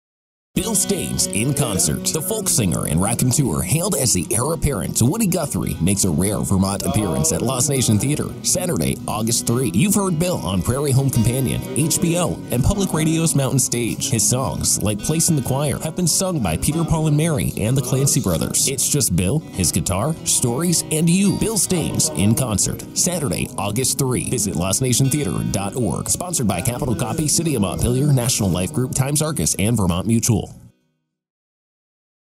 Bill Staines in Concert at LNT 2019!
Bill Staines in Concert
One Night Only - Sat Aug 3  7:30pm
Bill Staines– modern day folk music legend comes to Montpelier!

The beloved folkie Bill Staines comes to Lost Nation Theater for one night only. Bill has 26-Albums, numerous hits & TV & Radio appearances (including Prairie Home Companion) in his 41-year career, and his songs have been recorded by Peter Paul & Mary, The Clancy Brothers, and Nanci Griffith.  Staines has been hailed as the heir-apparent to Woody Guthrie.

“Bill Staines has been my hero since 1977. He carries on where Woody left off- carrying on the tradition of stories and characters you wish you knew.” (Nancy Griffith)